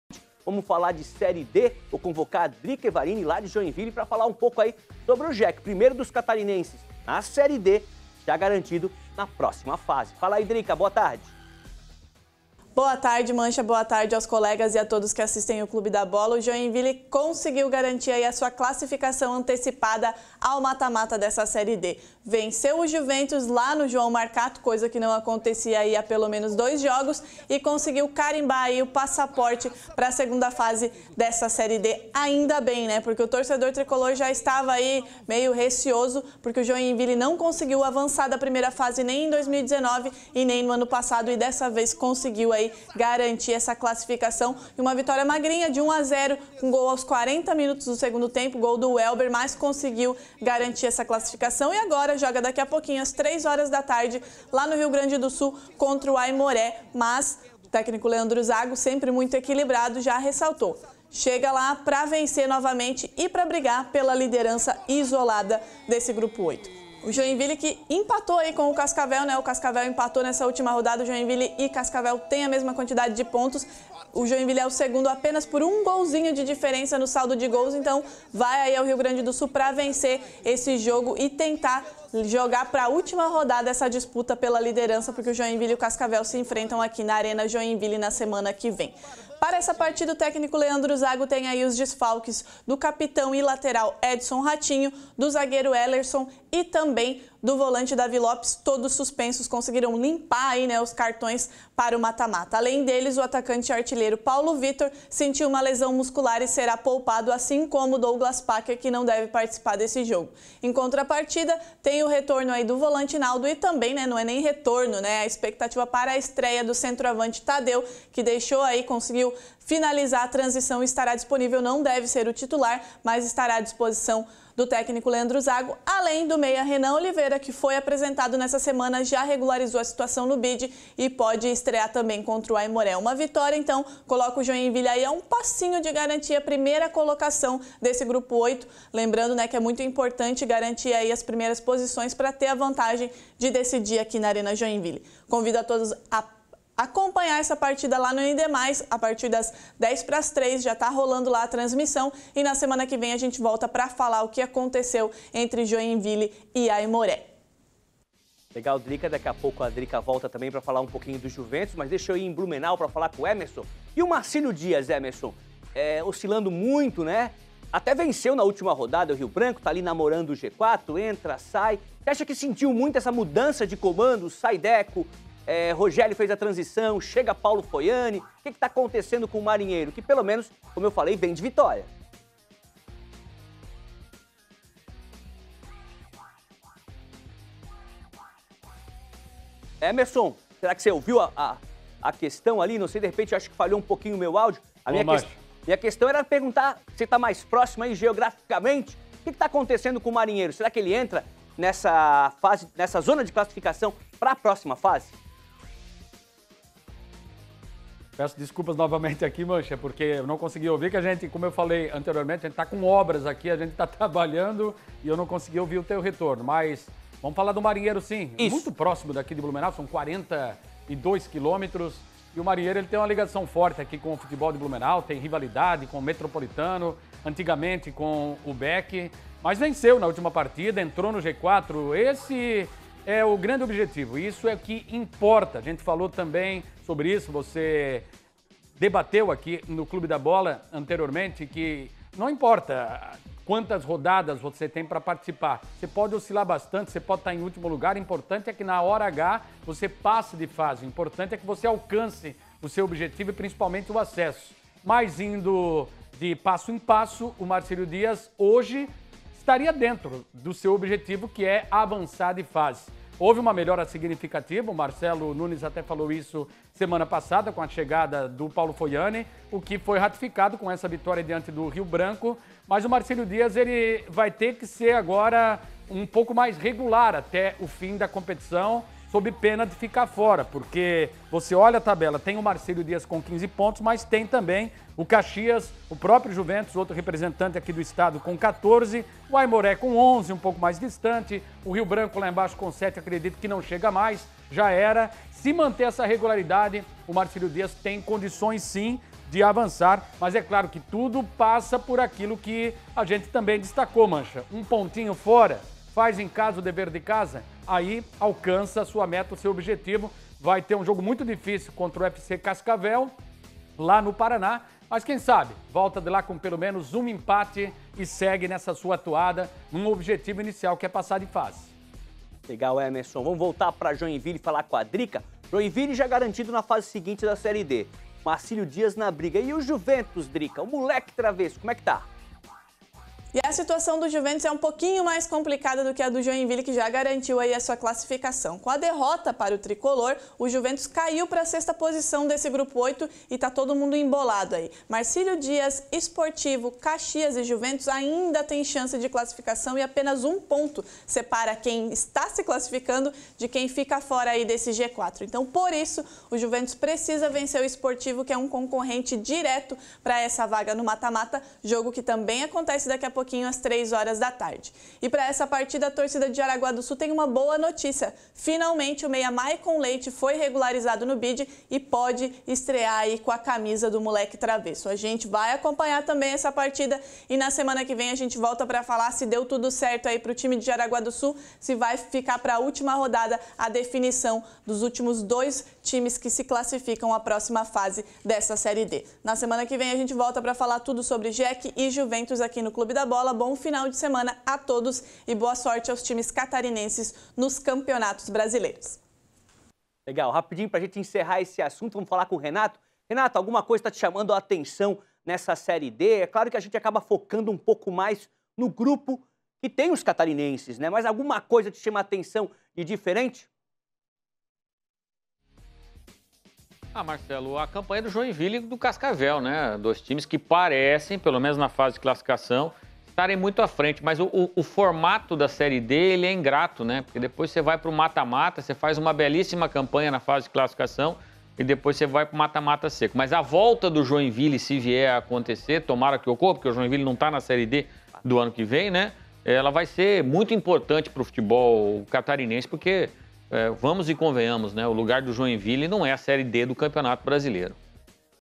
Série D: comentaristas analisam desempenho dos times catarinenses na competição
No programa Clube da Bola deste sábado, os comentaristas analisaram o desempenho do Joinville, Juventus e Marcílio Dias na Série D.

Veja!